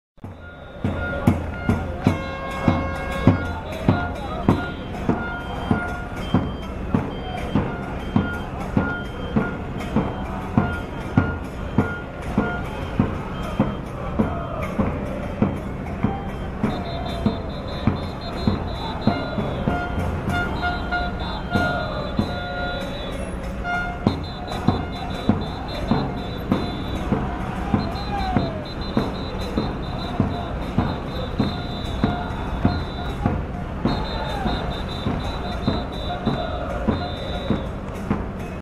Argentinos vibram nos arredores do Beira-Rio
Torcedores se aglomeraram para receber ônibus da seleção em frente ao estádio da Copa